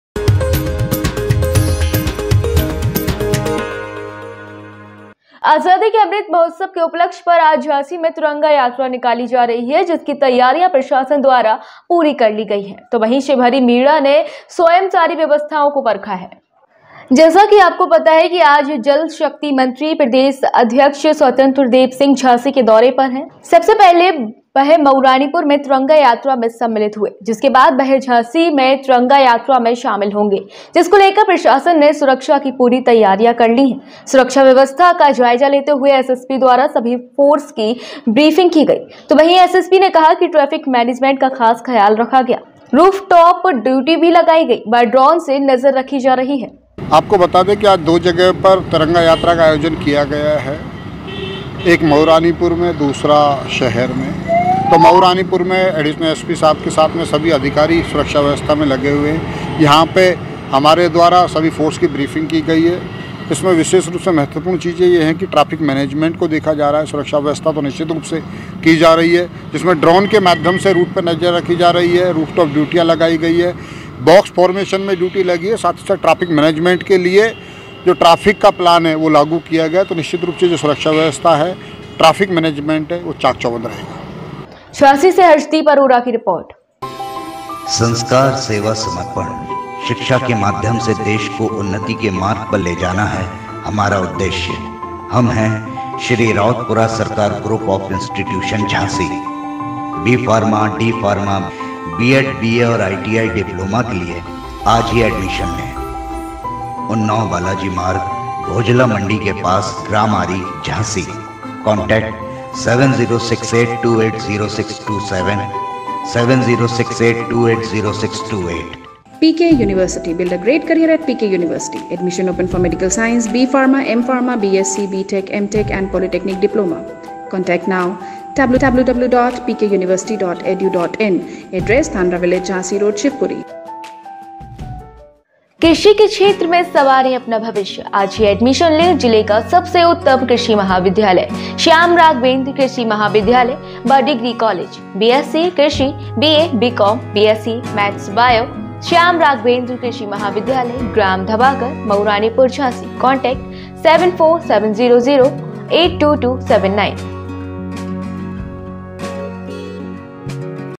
झांसी की तिरंगा यात्रा को लेकर एसएसपी ने देखी सुरक्षा व्यवस्था
Report : Harshdeep Arora